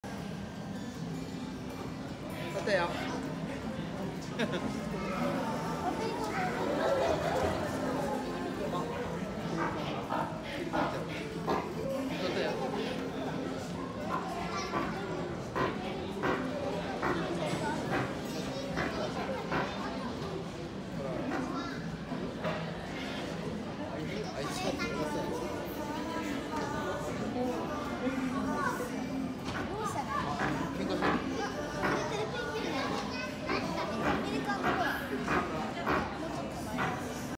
ニフレルのペリカンがペンギンを襲う
ニフレルにて、ペリカンがペンギンにかみついていました。